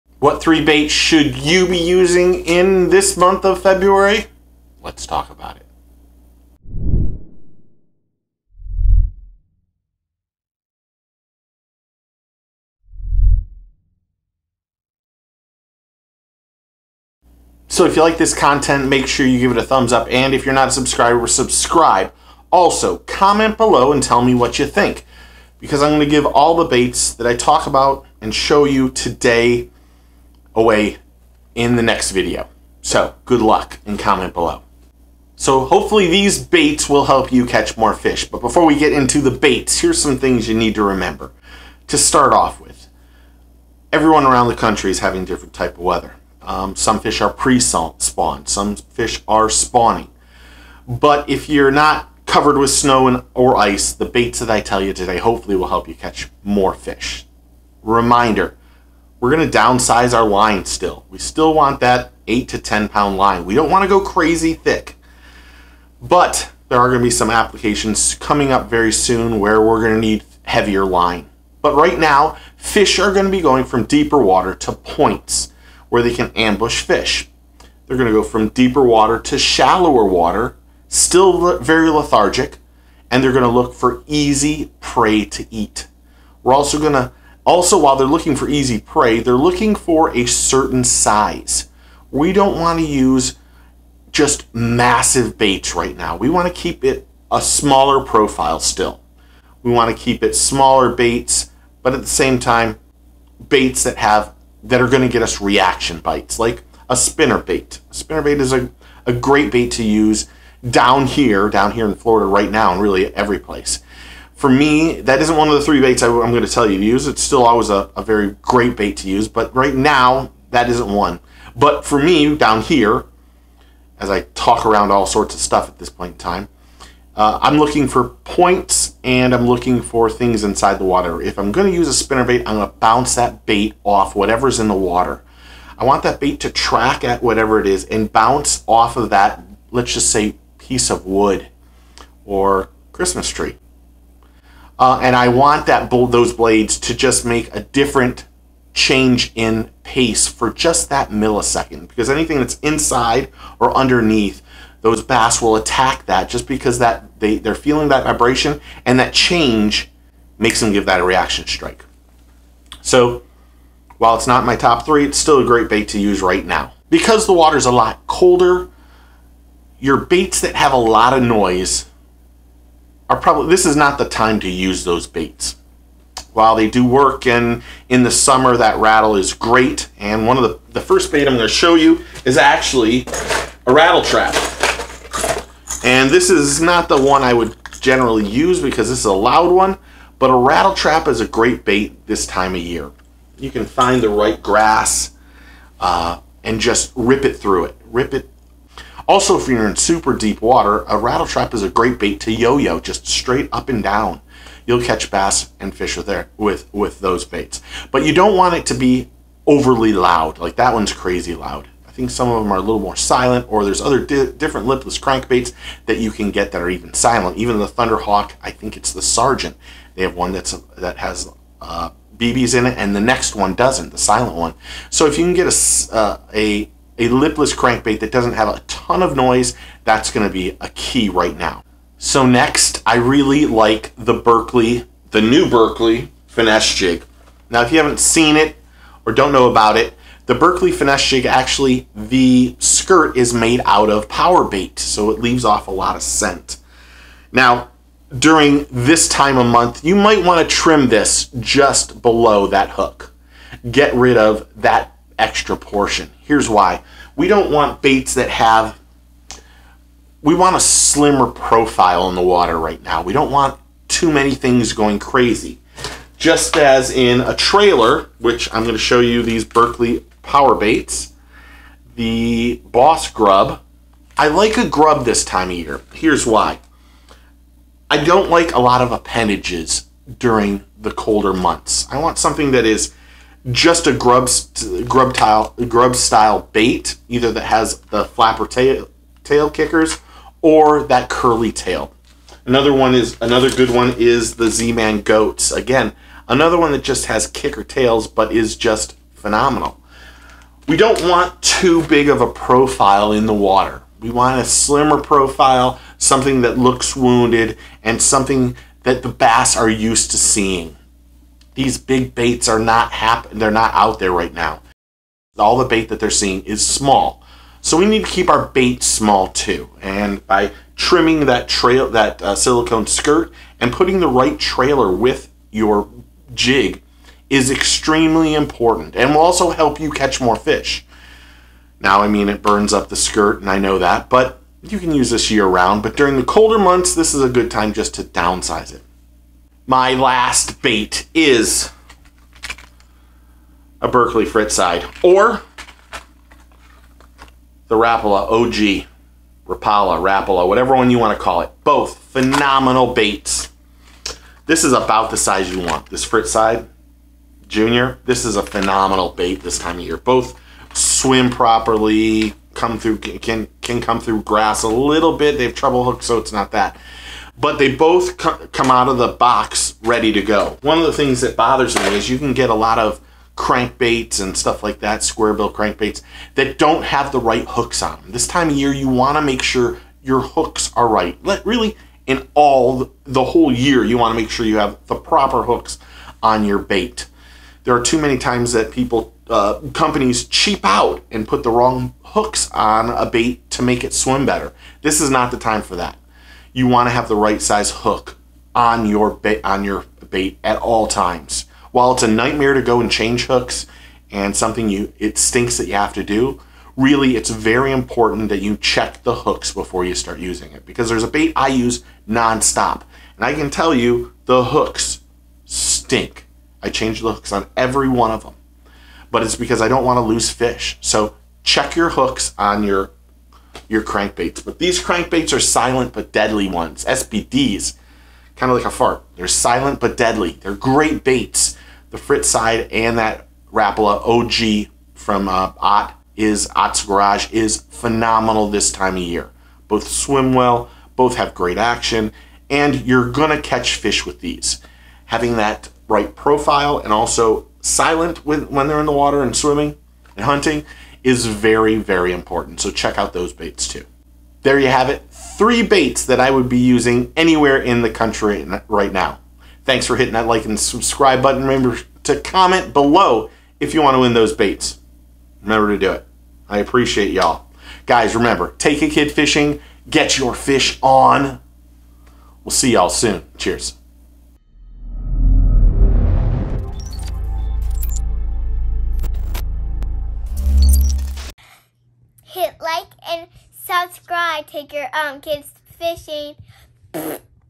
Three Bass Fishing Lures To Use in February To Catch More Fish
What three bass lures should you use to catch more predator fish and bass in February? Steve of Get Ur Fish On! talks about the month of February and what bass fishing baits and lures that you should be using to catch more fish.  From going with the silent approach to bass fishing lures, this should help you catch more fish during this month of February.